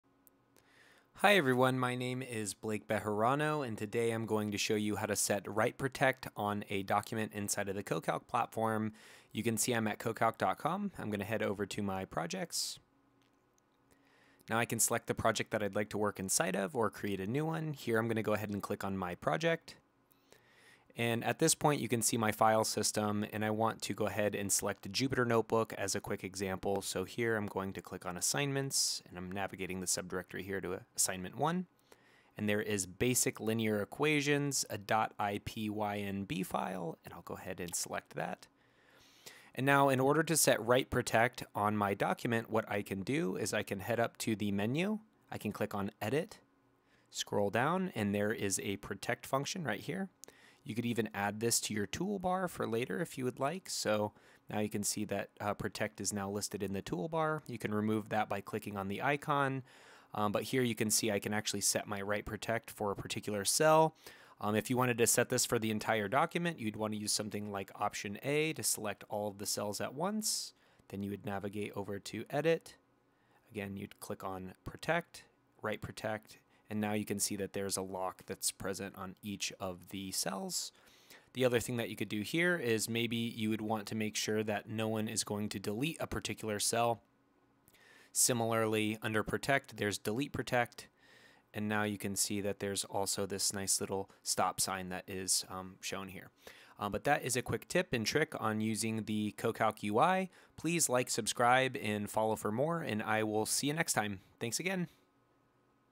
How to Access Write/Delete Protection in CoCalc
This concise tutorial teaches you how to efficiently access write/delete protection in CoCalc. We'll guide you through the process, ensuring that students, academics, and researchers can safeguard their work effortlessly. For more details, check the official CoCalc documentation